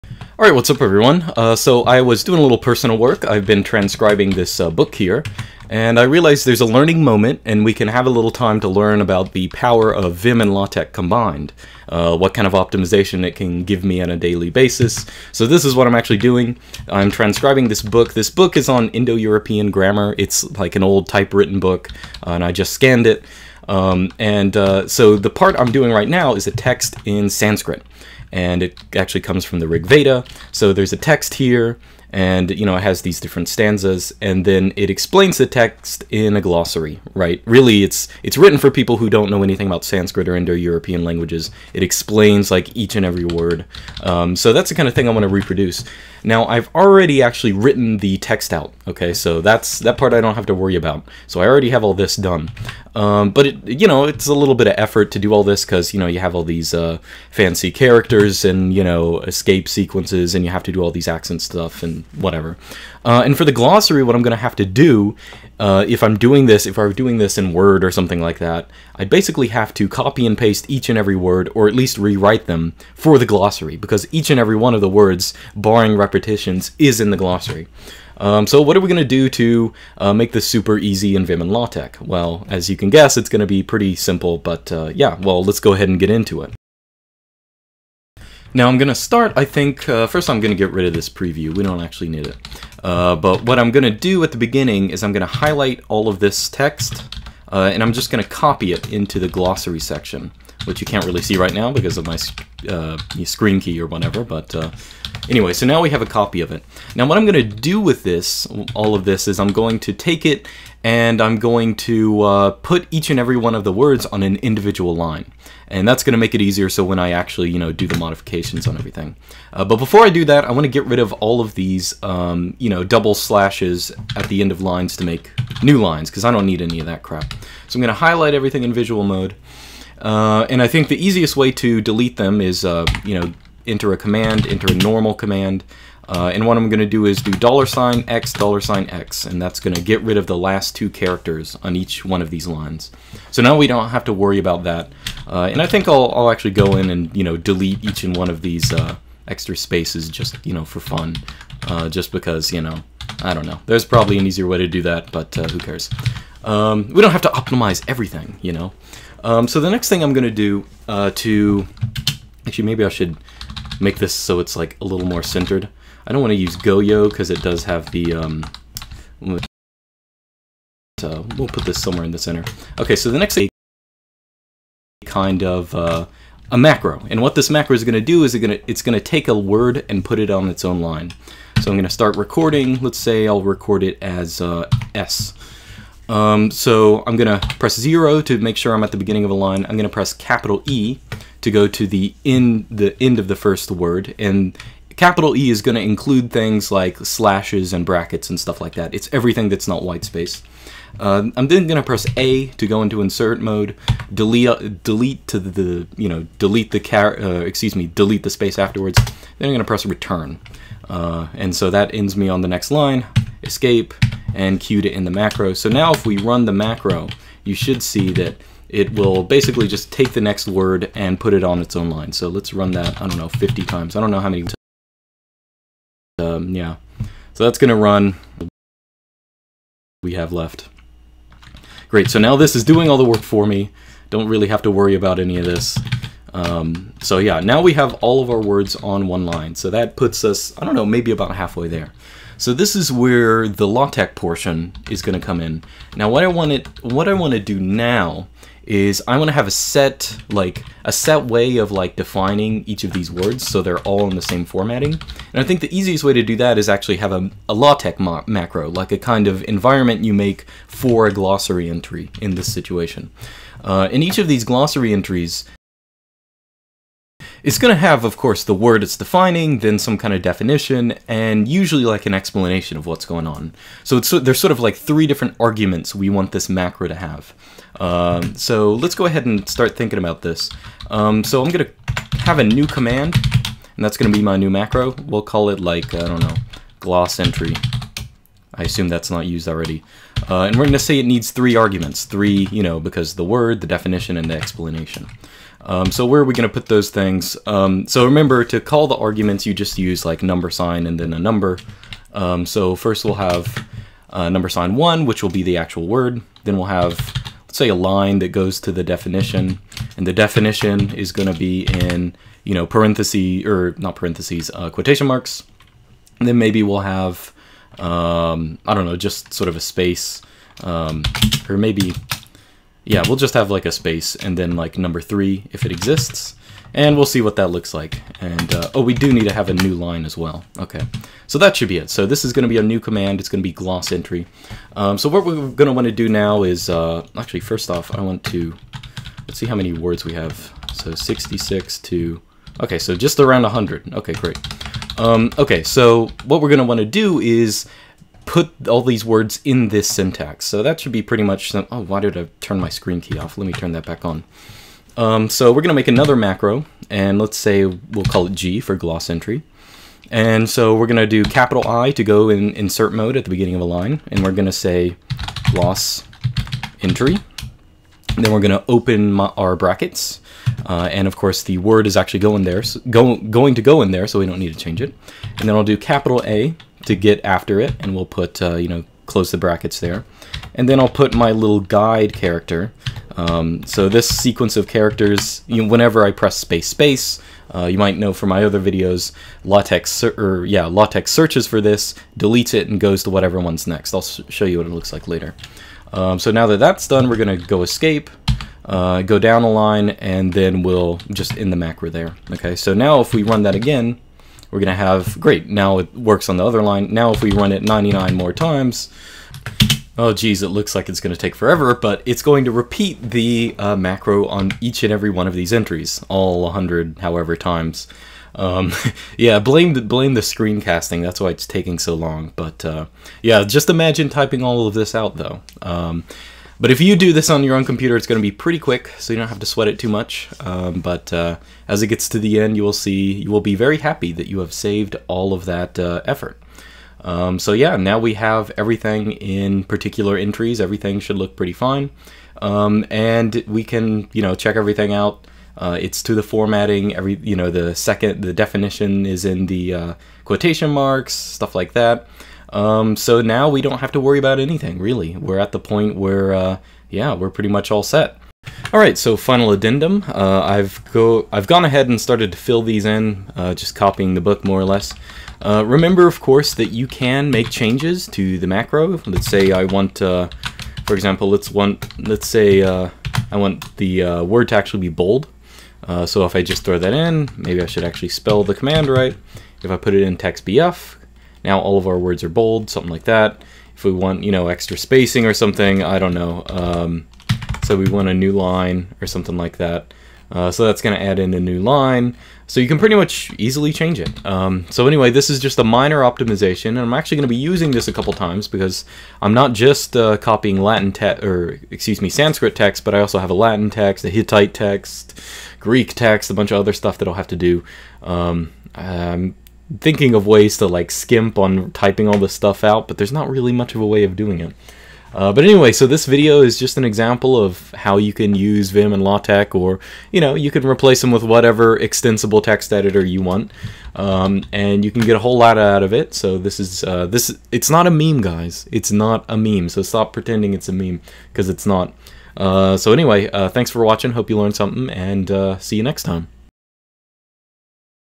vim and LaTeX do Ancient Rig Veda Wisdom together!
See the magic of my personal designated text editor and document compiler! I was transcribing a old type-written manuscript on Indo-European grammar, and realized there was a good opportunity for a learning moment to everyone interested in vim and LaTeX.